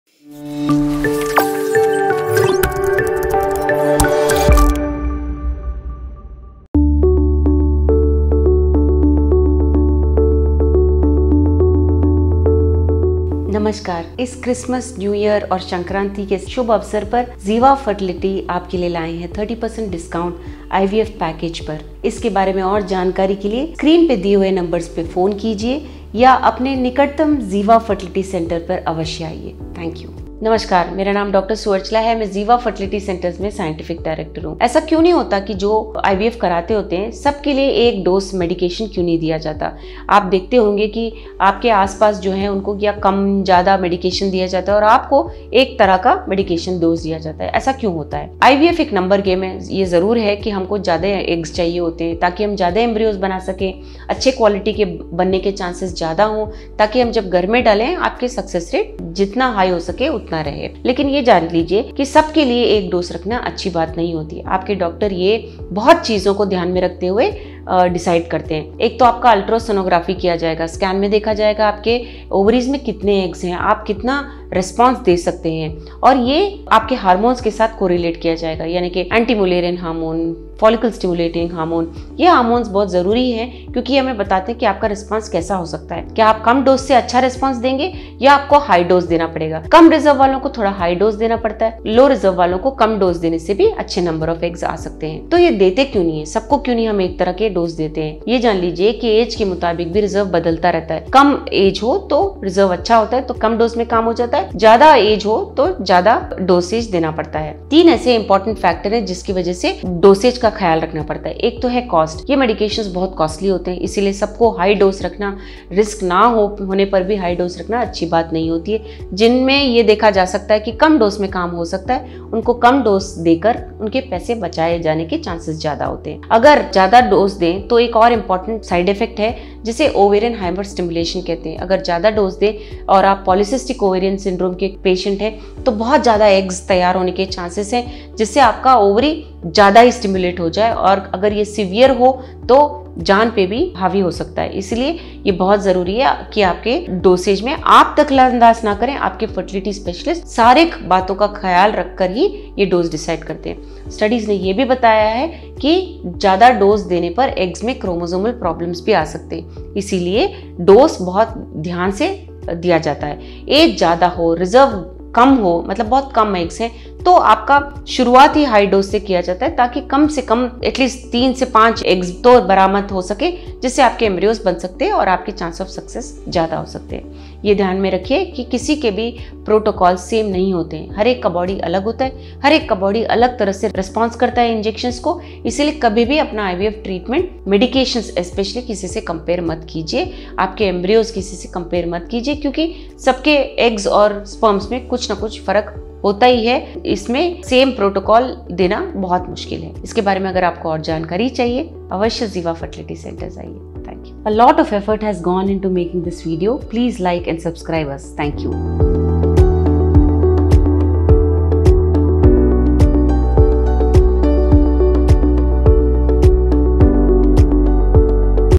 IVF Patients को एक जैसे INJECTIONS क्यों नहीं देते || Fertility Tips || Dr Chekuri Suvarchala
Dr. Chekuri Suvarchala (Scientific Director, Masters in Clinical Embryology (Australia & UK, PhD (Genetics)) - Topic: " Do high doses of IVF drugs cause harm ? " - ZIVA Fertility Hindi -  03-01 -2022

Ziva's Success story:
1)  22+ Years of Experience
2)  Around 90% Success Rate
3)  10,200 ICSI Cycles
4)  2,200 IVF Cycles
5)  4+ Centers in Hyderabad